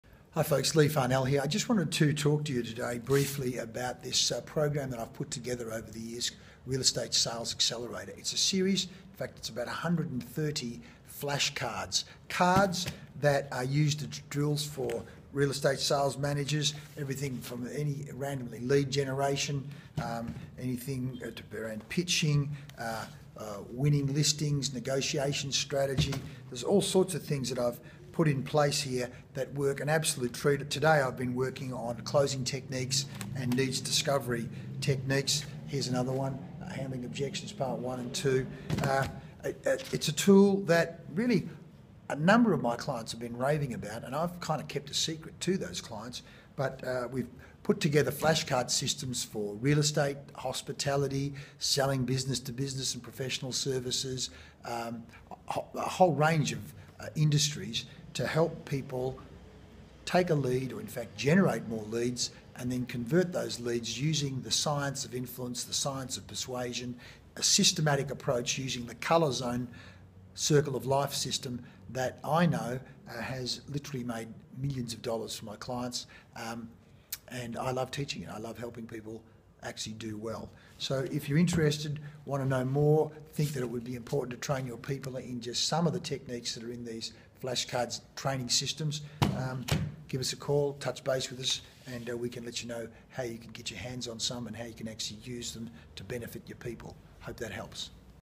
Copy of Sales Training Strategies to Fast Track Closing Success
For more Sales and Business Boosting tips and information go

Multi Award Winning Sales Coach Leigh Farnell outlines a powerful, proven training tool for Sales Teams based on an idea he got from his wife Catherine's Early Childhood Teaching class. Simple, easy, profound training tools used in Real Estate, Professional Services, Hospitality and Tourism.

Sign up for your FREE Leigh Farnell app and FREE gifts worth over $1200 from Leigh